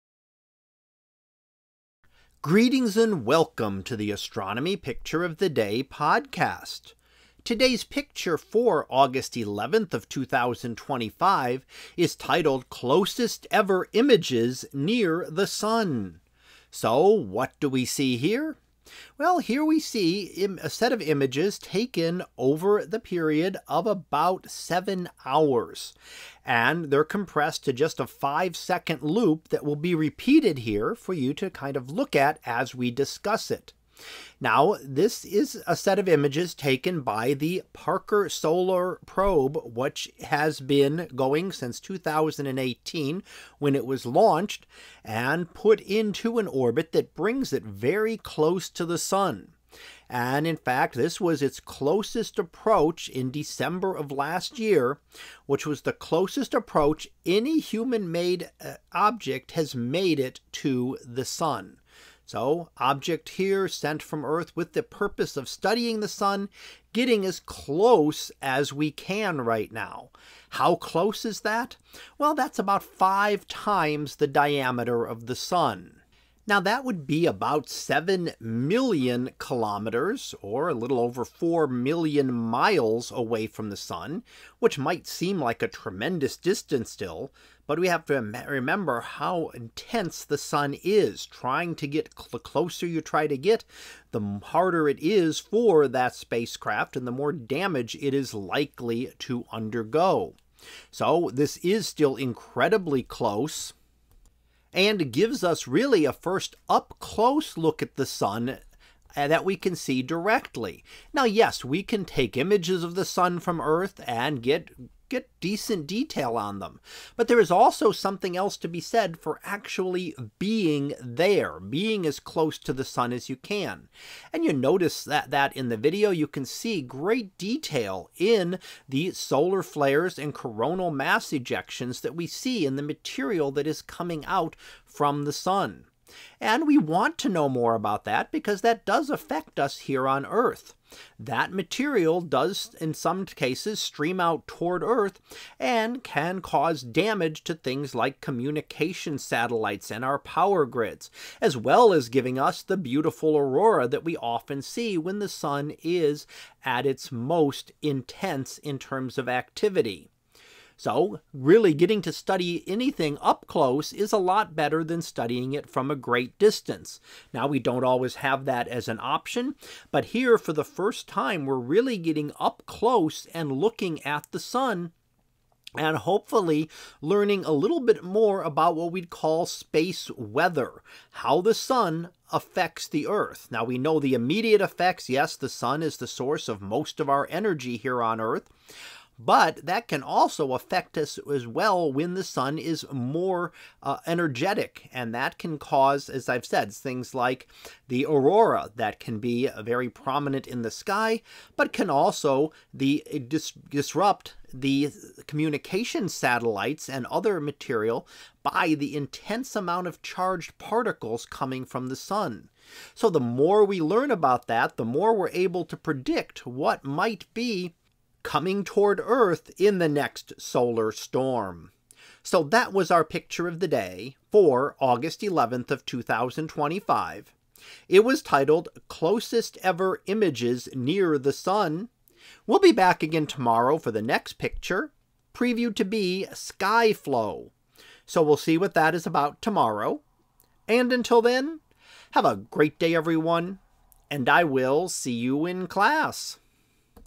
2025 August 11 - Closest Ever Images Near the Sun
In today's image, we see images from the Parker Solar Probe. These are the closest images of the Sun ever obtained, only a few solar diameters from our Sun's surface.